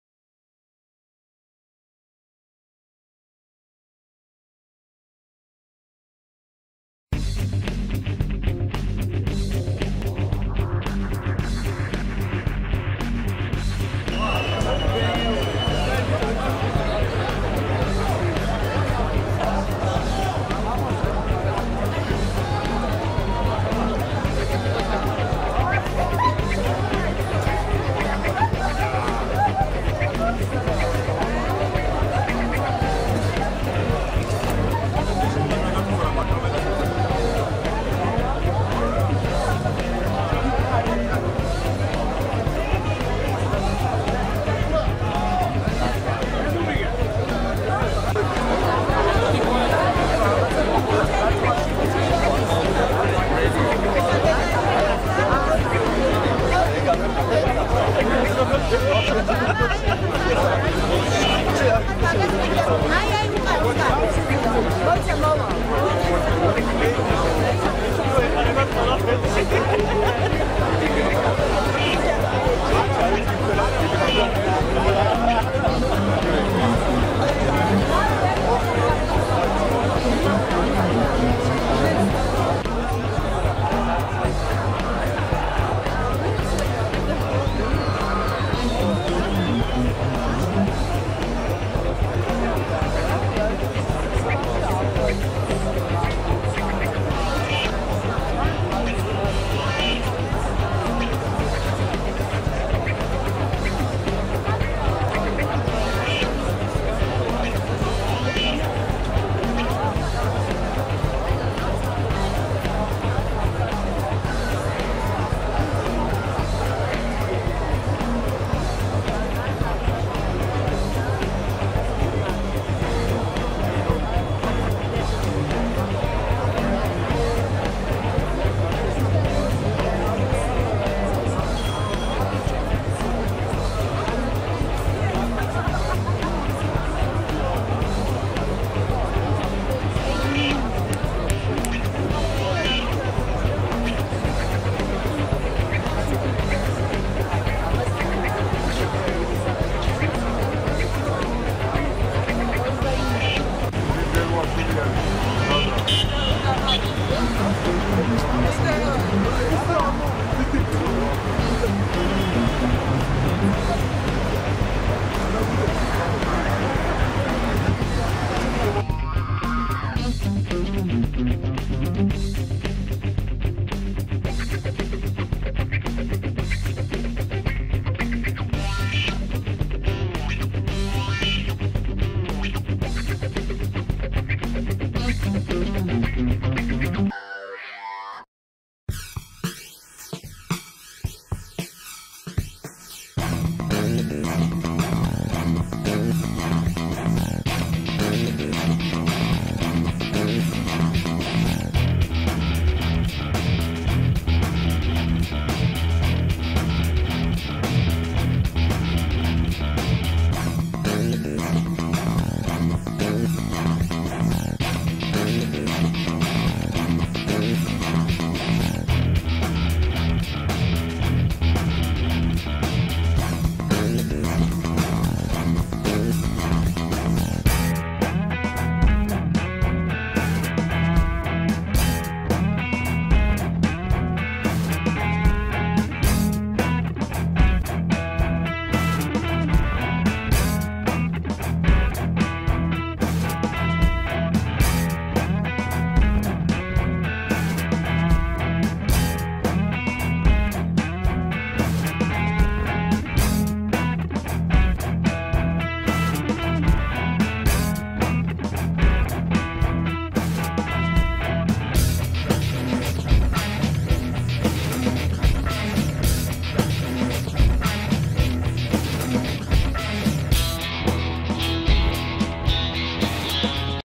'Say No to Theocracy' event Tbilisi, republic of Georgia May 24, 2013
'Say No to Theocracy' event Tbilisi, republic of Georgia May 24, 2013 for details on the story regarding May 17th, 2013 events in Tbilisi. My Ebay store On another topic - $17 a month membership - Legal Shield with profound benefits Music used in video presentation 'Say No to Theocracy' event in Tbilisi, republic of Georgia May 24, 2013 is